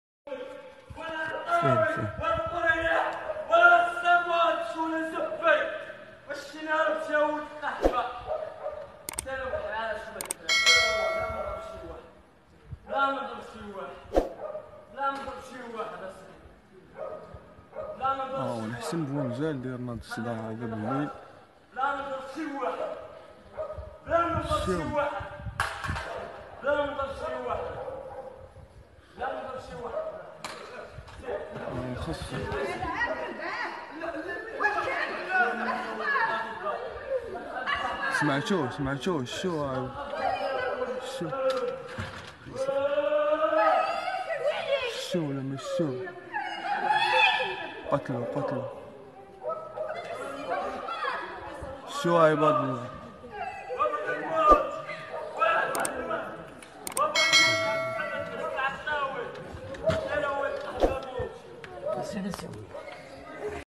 ڤيديو لأخطر مجرم (حسينة) بحي السلام الولفة الدار البيضاء الذي كان فارا من العدالة لكن تم القبض عليه
ڤيديو لأخطر مجرم حسينة بحي السلام الولفة الذي كان فارا من العدالة وخلف عدة ضحايا لكن تم القبض عليه ليلة السبت الماضي (الڤيديو للاسبوع الماضي)